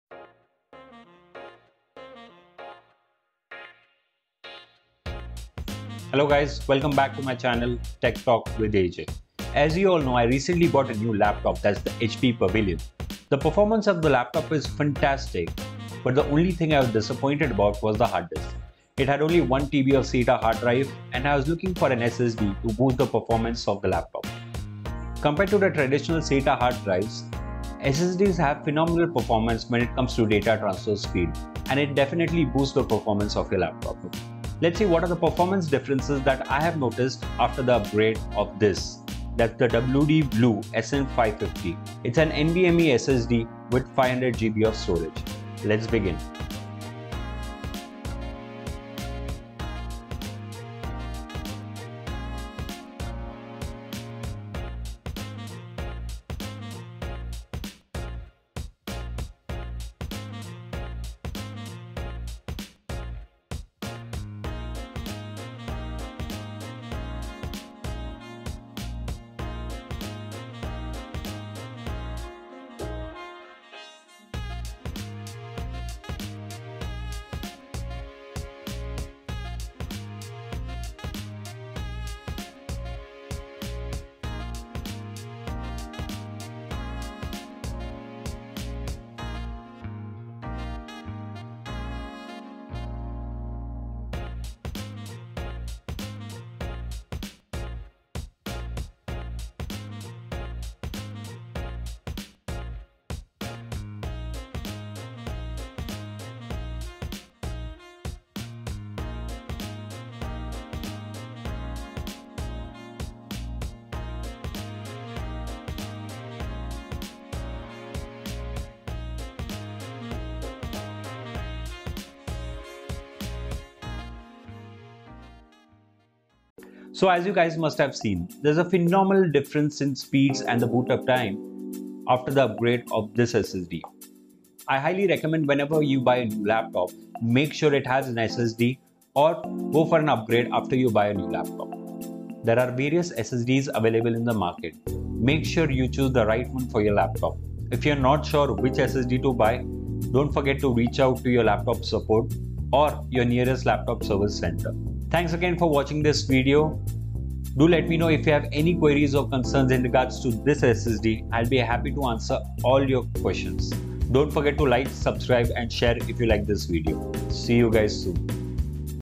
WD Blue SN550 NVMe SSD Upgrade to HP Pavilion 15-ec1024AX - Performance Test and Results
In this video I have upgraded the HP Pavilion 15-ec1024AX with the WD Blue SN550 NVMe SSD (500 GB) and compared the performance differences between the stock SATA Drive vs the WD Blue SN550 NVMe SSD.

Note:
1) Windows needs to installed on the SSD to see the performance difference in the laptop.
2) If you are not technical, please take your laptop to the nearest laptop service center to install the SSD and install Windows on the SSD.
3) There are various SSD drives available in the market and each laptop motherboard and BIOS support different SSD's. Make sure you buy the right SSD for your laptop.